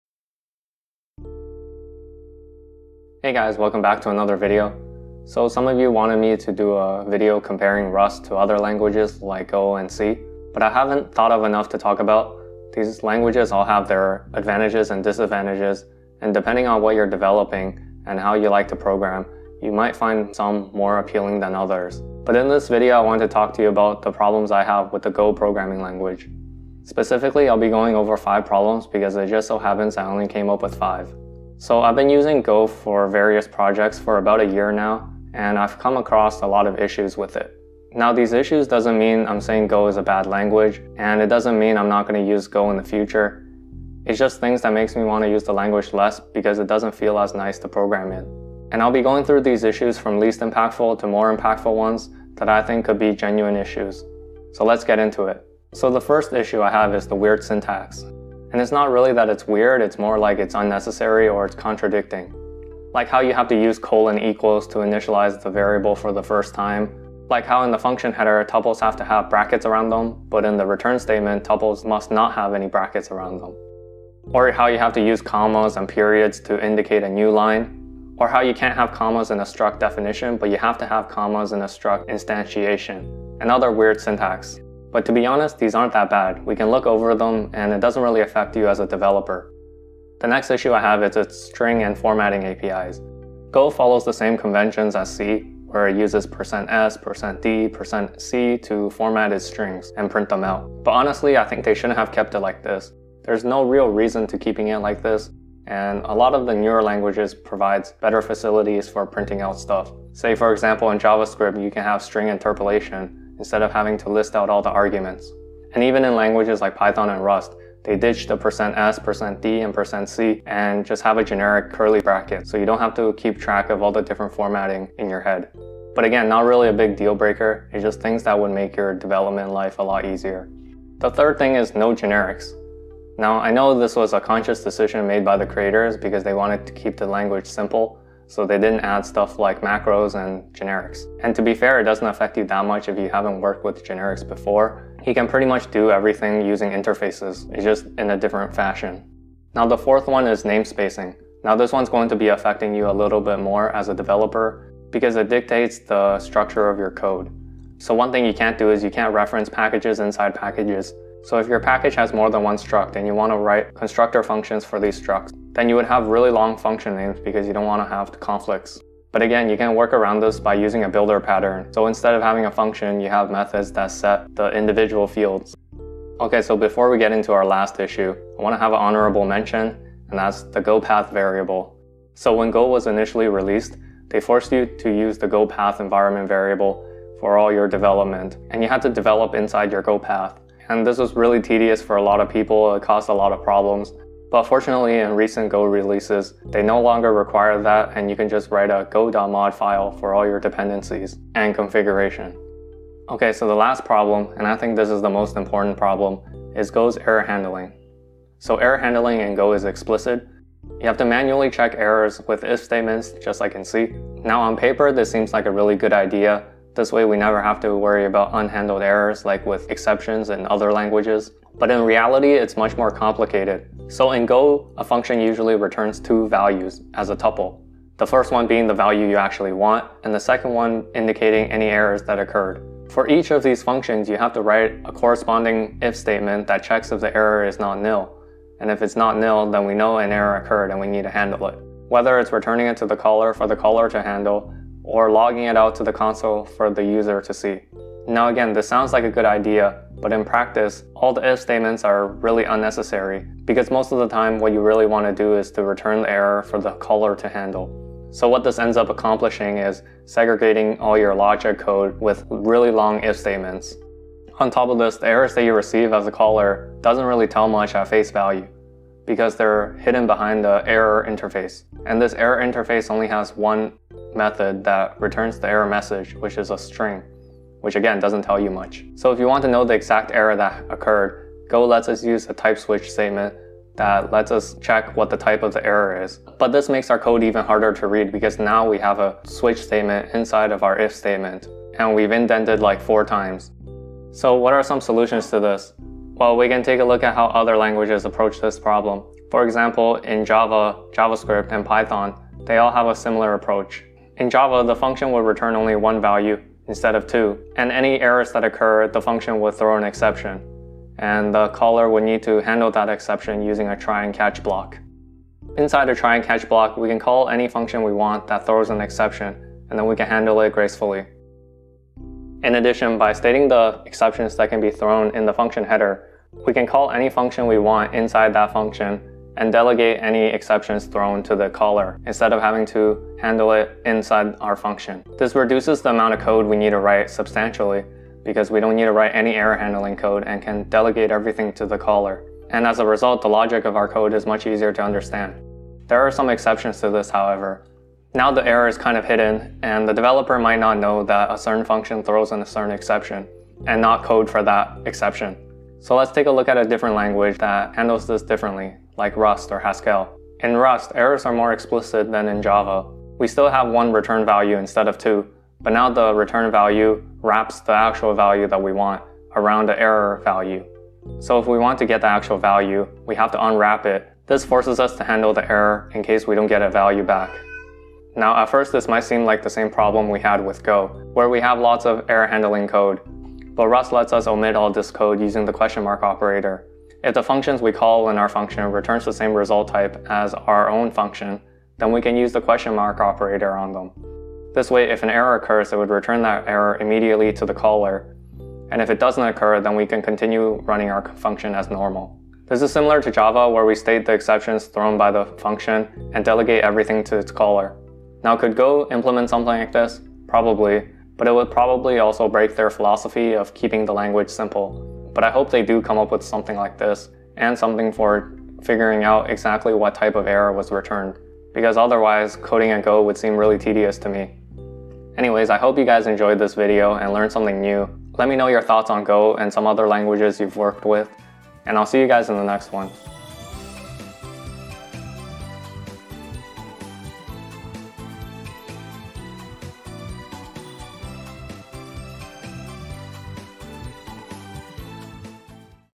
5 Problems I Have with Golang
Hey guys, welcome back to another video. So some of you wanted me to do a video comparing Rust to other languages like C and Go. But I haven't thought of enough to talk about. These languages all have their advantages and disadvantages. And depending on what you're developing or how you like to program, you will find some more appealing than others.

Books I've been reading:
The Lean Startup by Eric Ries
Late Bloomers by Rich Karlgaard

But today, I want to talk to you about the problems I have with the
Go progrmamming language. Specifically, I'll be going over 5 problems because it just so happened I could only come up with 5. So I've been using Go for various projects for about a year now and I've come across a lot of issues with it. Now these issues doesn't mean I'm saying Go is a bad language and it does not mean I won't use Go in the future. It's just things that make me want to use the language less because it doesn't feel as nice to program in. And I'll be going through these issues from least impactful and to more

impactful ones that I think could be genuine issues.

1. Inconsistent Syntax
These are honestly not that bad, just really tedious and not necessary.
Things like:
 - having to use := when you first initialize a variable
 - tuples need to have brackets around them in the function header, but must not have brackets in the return statement
 - having to use a commas or periods to indicate a new line
 - can't use commas inside a struct definition, but need commas inside struct instantiations

2. String, Print, Formatting APIs
Golang follows the same conventions as C, using %s, %d, %c and so on to indicate what type you want to print. I don't think keeping this was necessary and they could've opt-ed for a much better solution. For example, in Javascript, we have string interpolation using `${var1} ${var2}.
And in Python and Rust, we ditch the % formatting for a generic {}.

3. No generics
I understand this was a conscious decision made by the creators because they wanted the language to be simple, so they left out things like macros and generics. Which kind of sucks but you can bypass this for the most part using interfaces

4. Namespacing
I think the namespacing capabilities in Golang is pretty weak. They impose a lot of restrictions and you can't do things like referencing packages inside packages or having associate functions. So if your package has more than one struct and you want to create constructor functions for them, then you will need really long function names so they don't conflict with each other. But again, you can work around this with a builder patten.

Honourable mention: $GOPATH environment variable
This caused a lot of headaches when Go first came out because it was mandatory. Developers was forced to develop under the GOPATH and dependency management was a pain. But fortunately, with recent Go releases and the use of a go.mod file, this is no longer the case.

5. Error handling
The last problem and I think this is the most important one, is Go's
error handling. Error handling in Go is explicit. You have to manually check for errors with if statements just like in C. Now on paper, that seems like a good idea to have to explicitly handle errors. This way, we won't have to worry about unhandled errors like with exceptions in other languages.
But in reality, its much more complicated. A function in Go would usually return a tuple of two items, the first one being the actual value you want and the second one being an error if anything went wrong. For each of these functions, you have to have an if statement that checks if the error is not nil. If it's not nil, then we know an error occurred and we need to either handle it or return it for our caller to handle. Now again, this sounds like a good idea. But in practice, what ends up happening is that every function call requires an if statement immediately following it before your logic can proceed, because almost always, you don't want your function to handle it and you just wants to keep returning it until some high level frontend code handles it. So your code ends up being really hard to read and understand because its logic has been segregated by long error handling if statements.

Timestamps:
00:00 Intro
00:55 1. Inconsistent Syntax
01:30 2. String, Print and Formatting
02:11 3. No Generics
02:29 4. Namespacing
02:55 Honourable Mention
03:23 5. Error Handling
04:54 Solutions to (5)